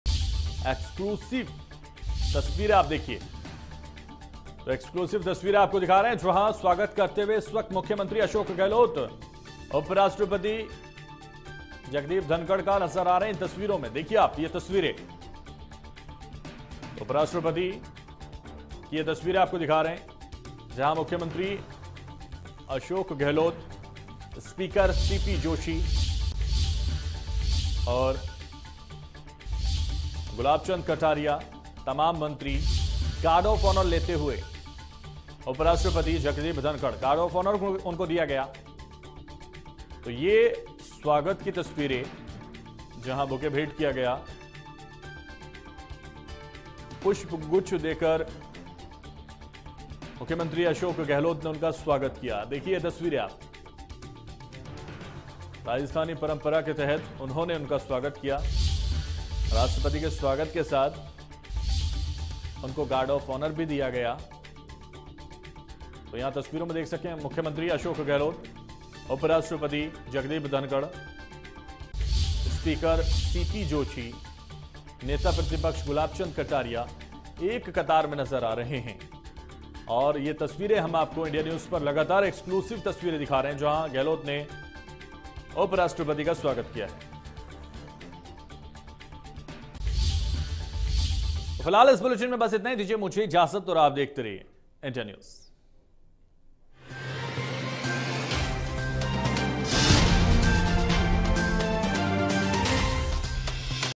Rajasthan News : उपराष्ट्रपति जगदीप धनखड़ पहुंचे विधानसभा | Hindi News
Rajasthan News : उपराष्ट्रपति जगदीप धनखड़ पहुंचे विधानसभा,स्पीकर सीपी जोशी ने भी किया स्वागत | Hindi News